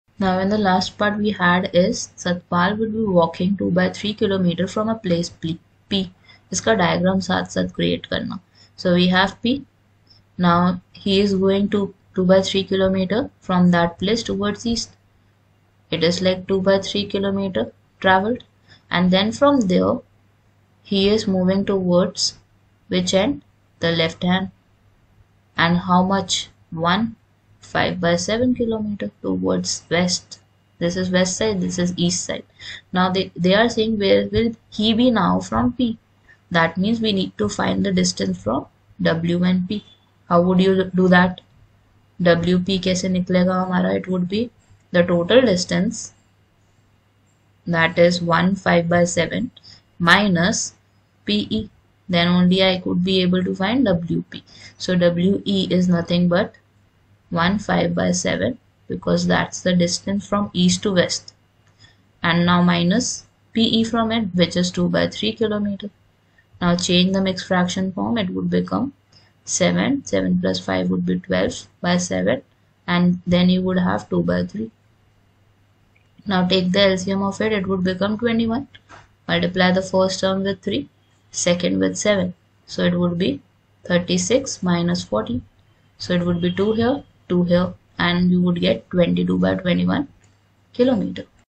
6. Satpal walks 2/3 km from the point P, towards east and then from there 1(5/7) km towards west.
6. Satpal walks 2/3 km from the point P, towards east and then from there 1(5/7) km towards west. Where will he be now from P?